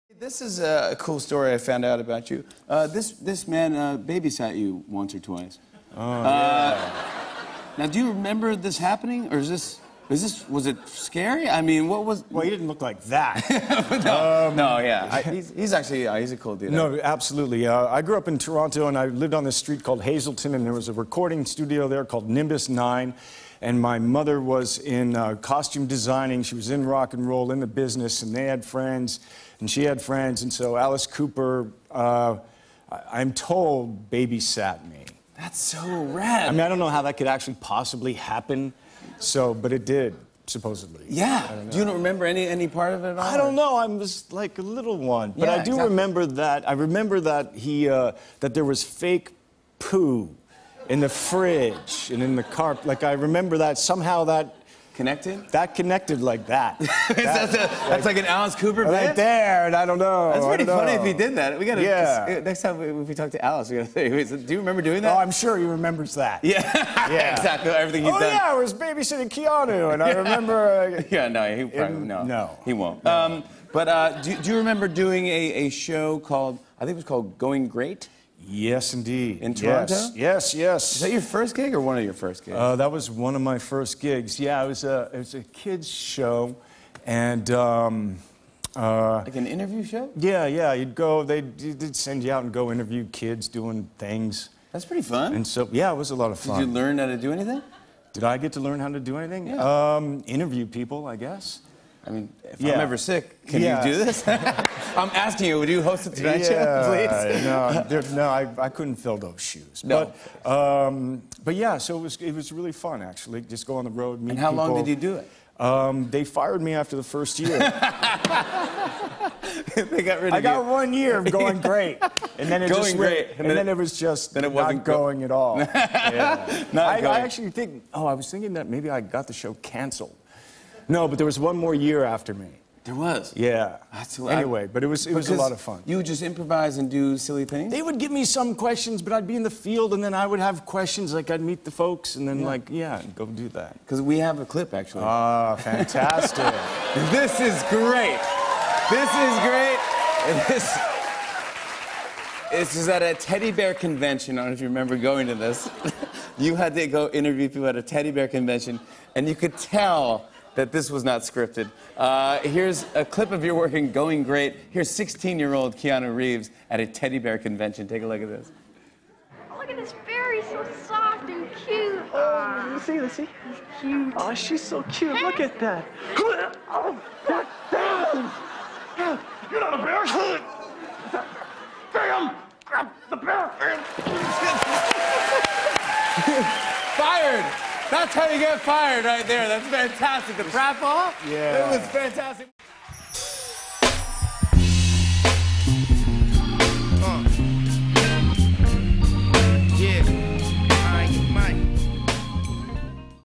Alice Cooper Used to Babysit for Keanu Reeves in Canada
Keanu Reeves discusses being babysat for by a young Alice Cooper and shows footage of the cheeky improvising that got him fired from a Canadian kids show after one year.